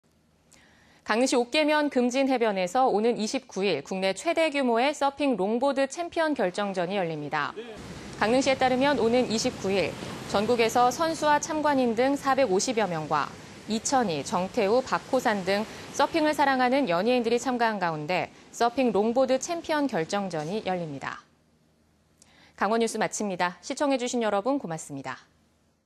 [단신]강릉 금진해변에서 서핑 롱보드 챔피언 결정전 180926
강릉시 옥계면 금진해변에서 오는 29일 국내 최대 규모의 서핑 롱보드 챔피언 결정전이 열립니다.
           ◀ＥＮＤ▶
 강릉시에 따르면 오는 29일 전국에서 선수와 참관인 등 450여 명과 이천희, 정태우, 박호산 등 서핑을 사랑하는 연예인들이 참가한 가운데 서핑 롱보드 챔피언 결정전이 열립니다.

 강릉시는 금진해변을 서핑 특화 해변으로 만들기 위해 최근 군사 시설물을 철거하고 백사장 평탄 작업을 진행한 데 이어 서퍼들을 위한 편의시설을 꾸준히 확충하기로 했습니다.